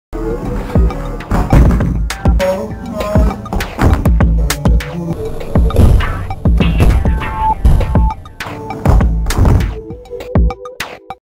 Kids Underfoot
a few lame tricks while taking care of my kids